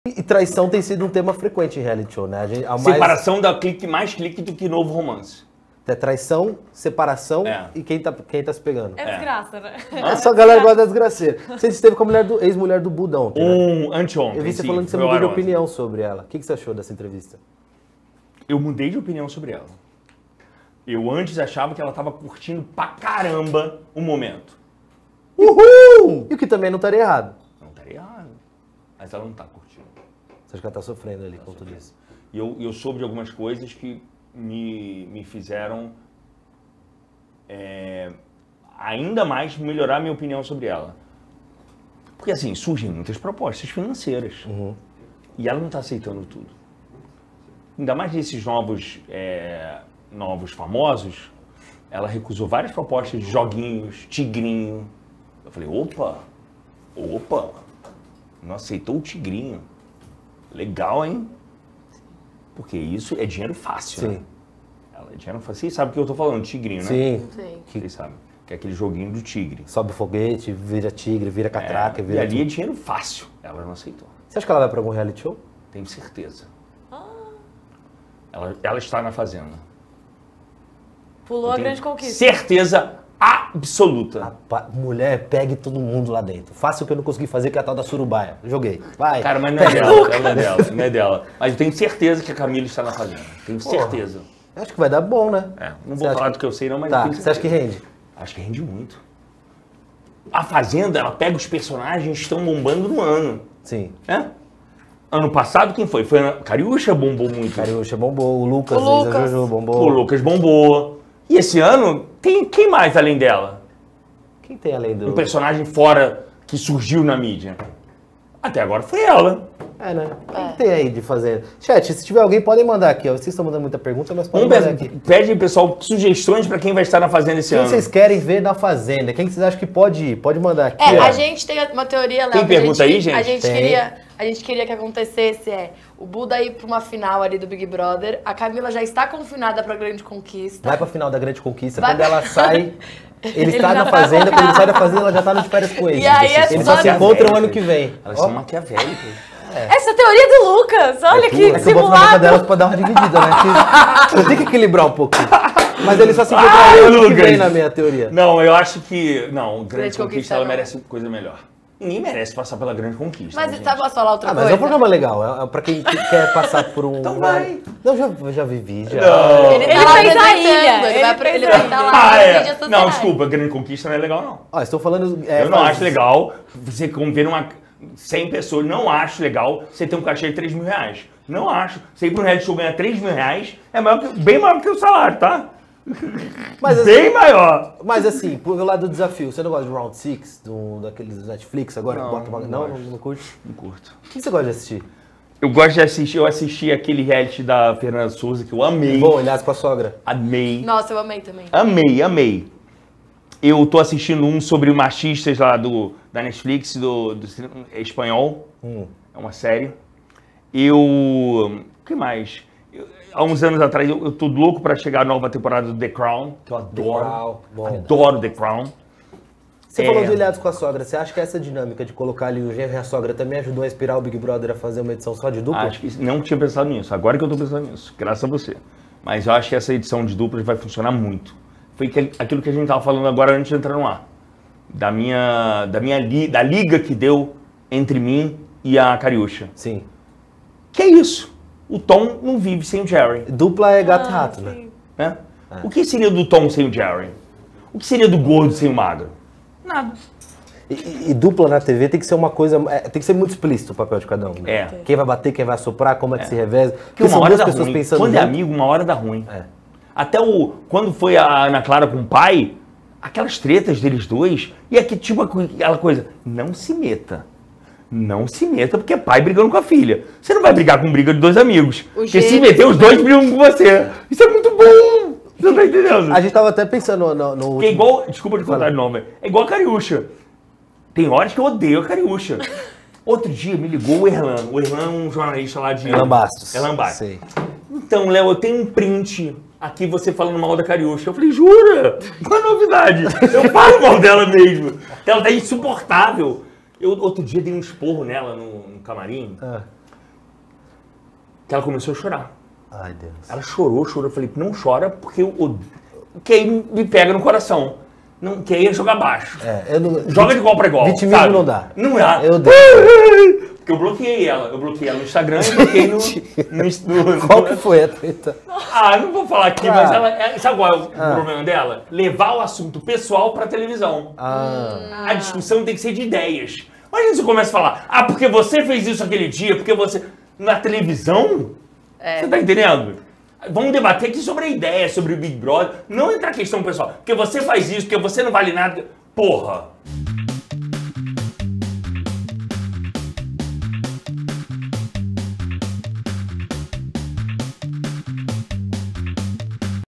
LEO DIAS AFIRMA QUE CAMILA MOURA, EX DE BUDA, ESTARÁ EM A FAZENDA 16
O jornalista cravou a presença de Camila Moura, que já soma mais de 3 milhões de seguidores no Instagram, na próxima edição do reality da Record.

Gabi Augusto - @gabiaugusto
Dezza Tornelli - @dezzatornelli
Henrique Mathias - @henriquemathias
Caio Barbosa - @caioobar